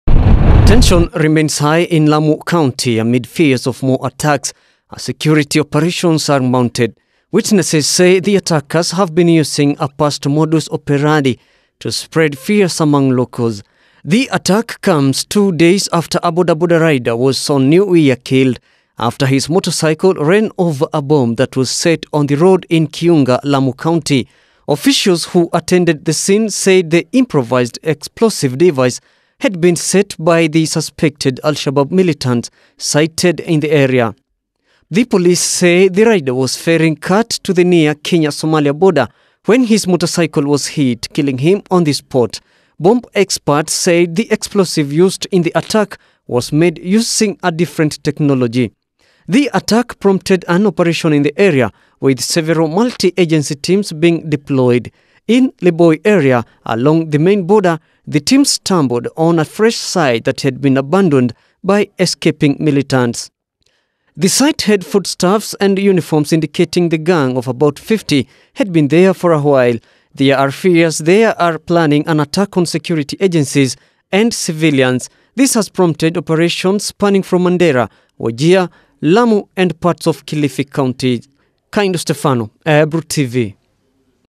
Al-Shabaab Dawn Attack On Lamu Village
At Least Six People Were This Morning Killed Imn What Police Say Is   An Attack By Al-Shabaab Militants In The Widhu Majembeni Area In Lamu County. Police Said The Suspected Militants Also Torched Several Houses In The Attack Before Escaping. According To The Police And Survivors, One Of The Victims Was Shot Dead, Another Was Hacked To Death While The Other Four Were Burnt To Death In Their House. Lamu County Commissioner Irungu Macharia Confirmed The Incident Saying An Operation To Get The Attackers Was Ongoing. The Attack Was Anticipated By Security Agencies Who Said The Gang Had Been Sighted In The Larger Boni Forest And Was On The Move.